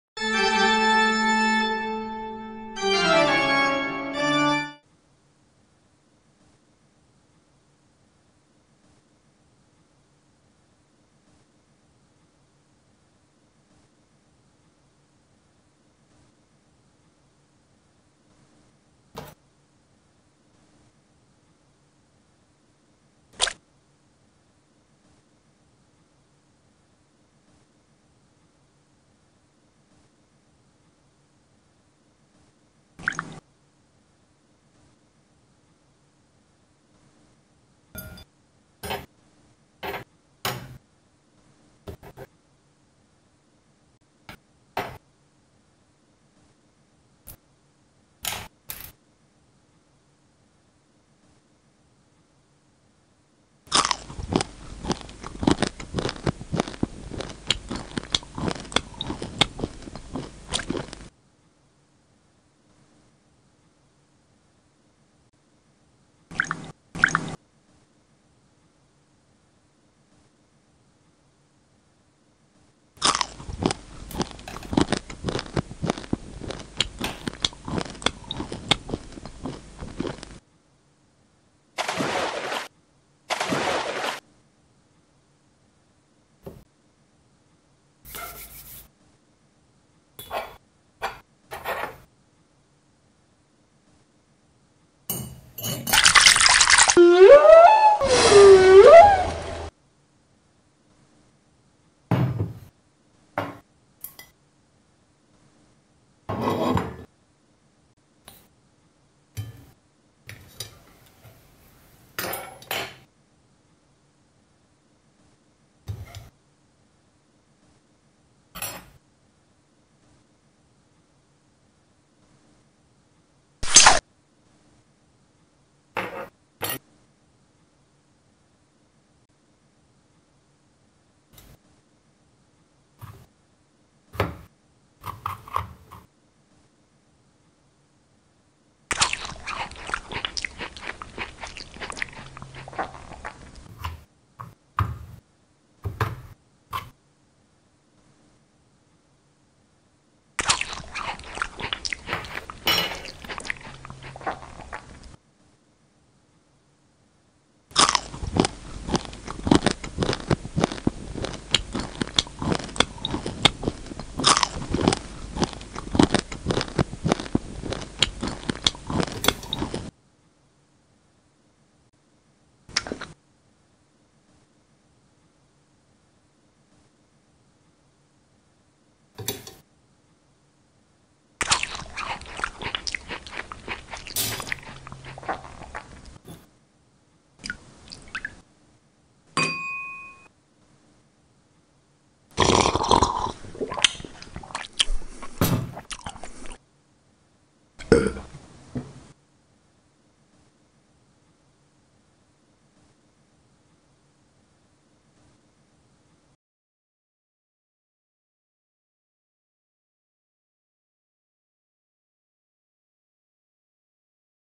Kluna Tik Incredible Whale Fishing vs Eggs ! Kluna Tik Style Dinner #39 | ASMR eating sounds no talk
➜ Hi, I'm Kluna Rice ASMR and together with my venus flytrap (Name : Rice ) we eat funny/absurd meals like: Hulk, soap and much more!